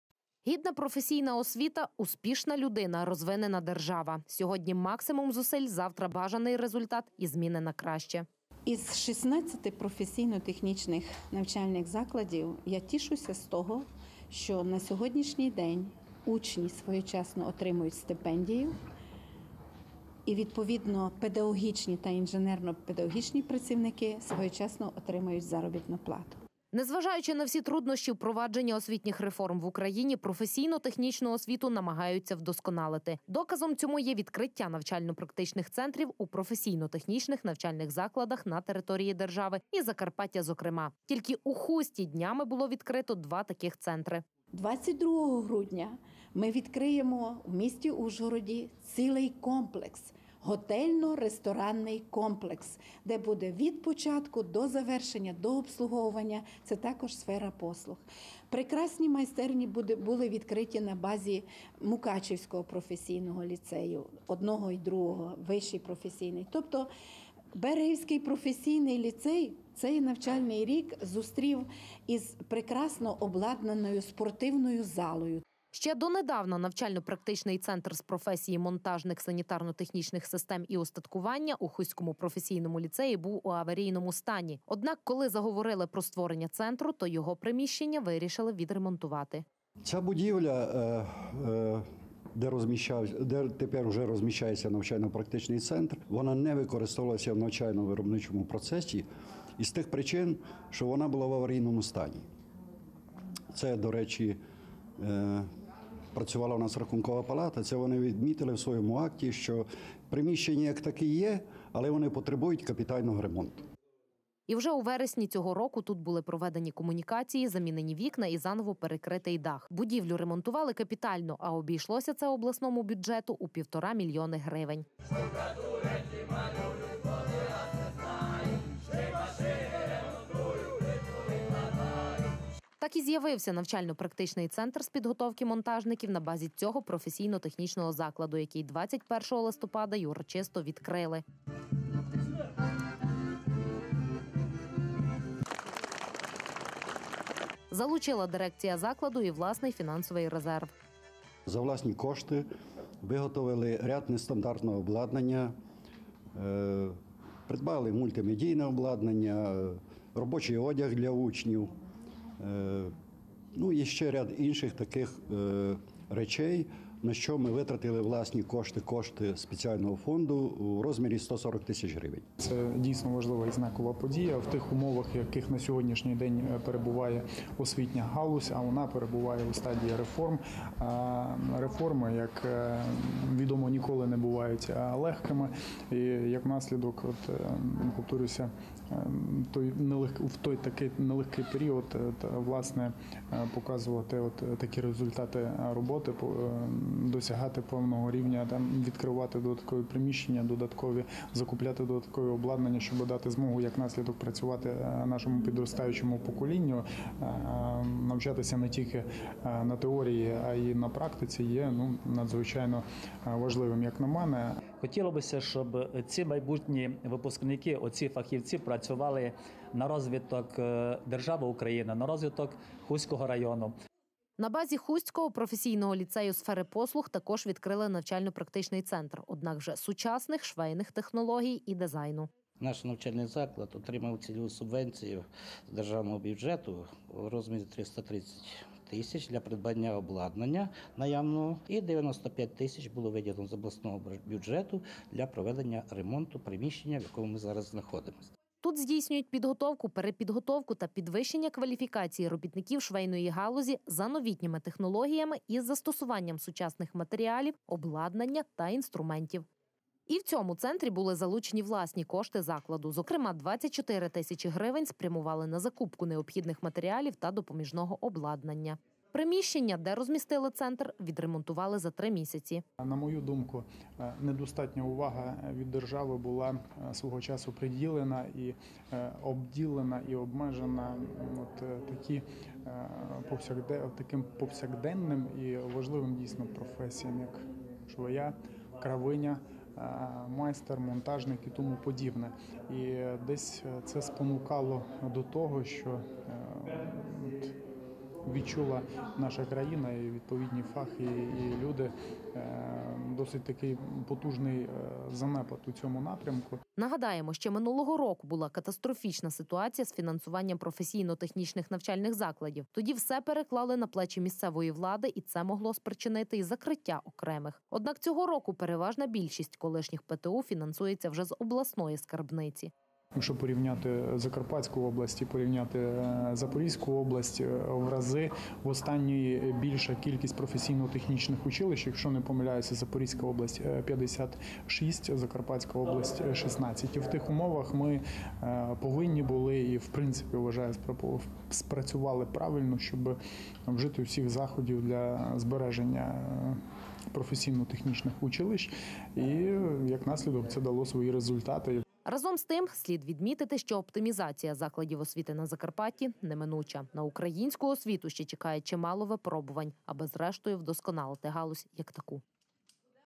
Розвиток професійної освіти
Попри усі труднощі по впровадженню освітніх реформ професійно – технічна освіта розвивається. Доказом цьому є відкриття додаткових та осучаснених приміщень для виробничої практики. У листопаді в області відкрили два навчально – практичні центри на базі професійно – технічних закладів. І обидва у ХУсті.